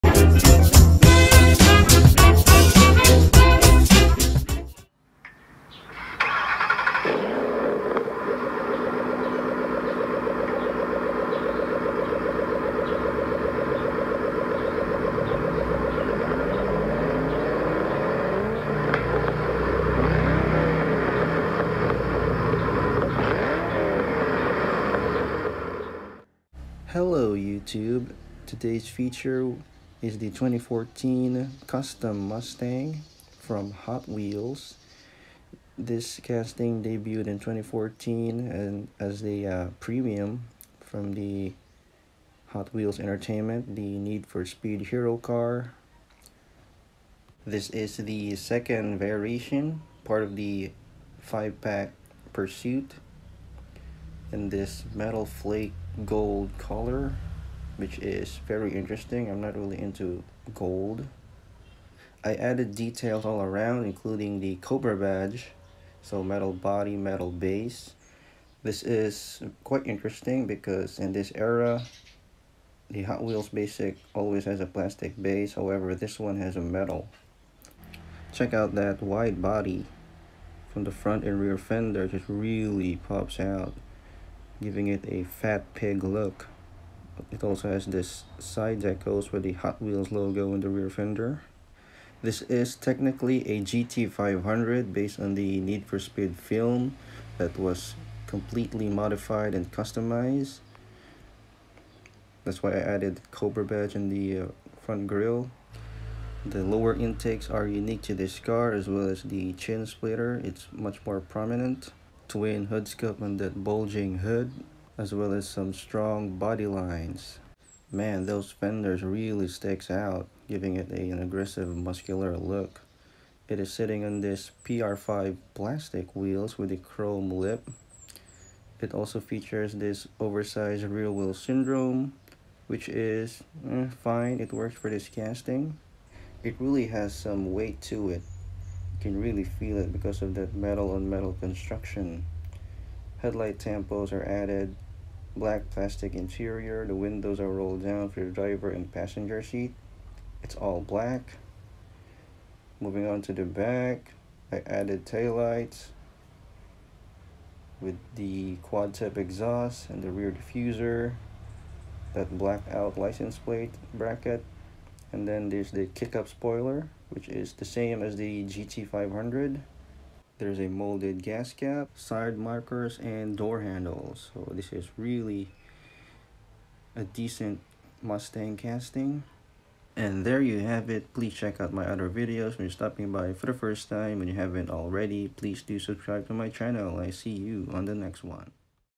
2014 Custom Mustang Hot Wheels
This Mustang is from a pursuit 5 pack in 2015. Metal body and metal base which is interesting. First debuted in the Need For Speed film in the Hot Wheels Entertainment.